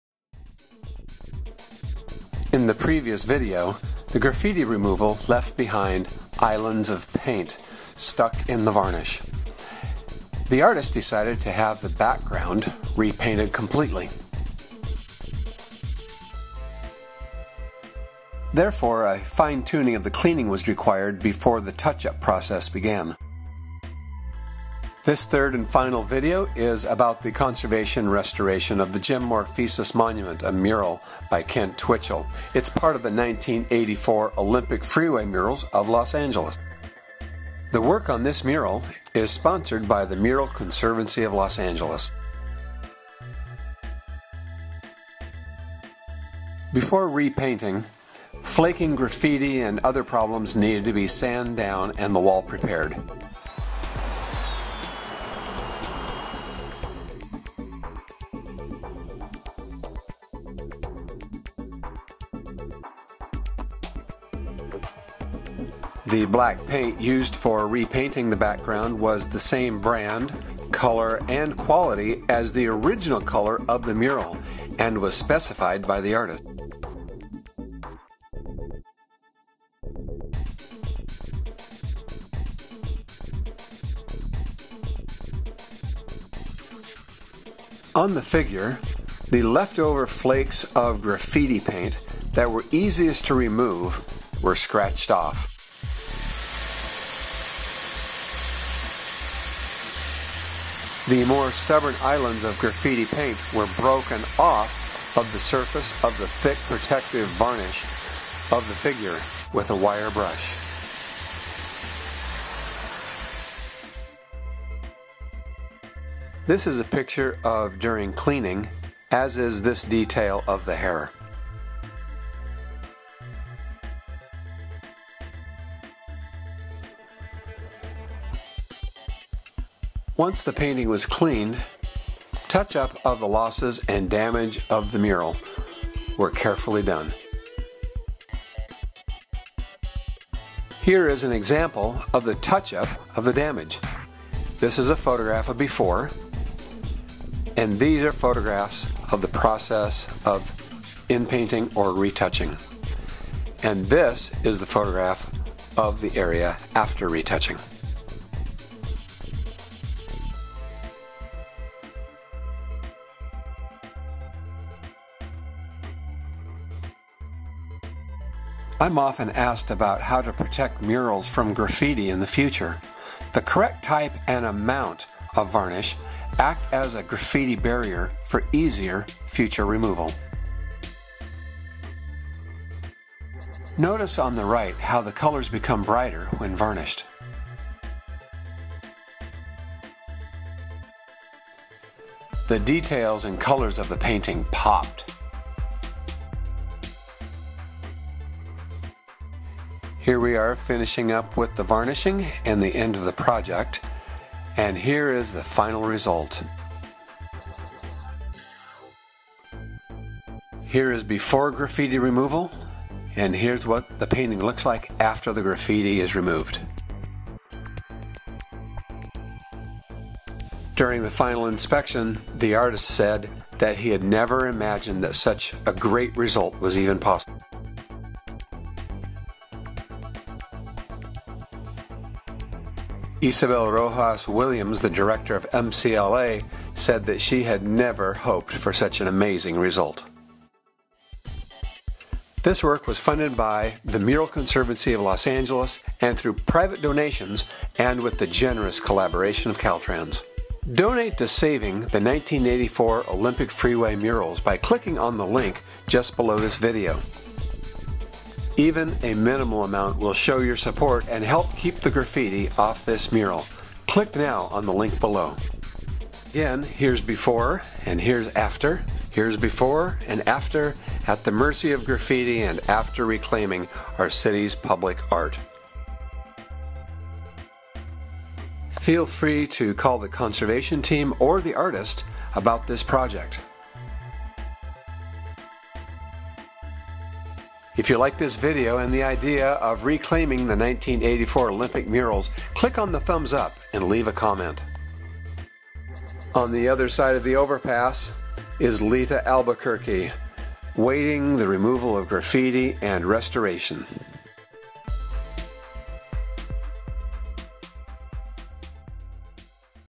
Mural Restoration of Jim Morphesis Final Video - 1984 Olympic Freeway Mural Downtown LA
Please call MCLA to donate to help keep graffiti off the mural.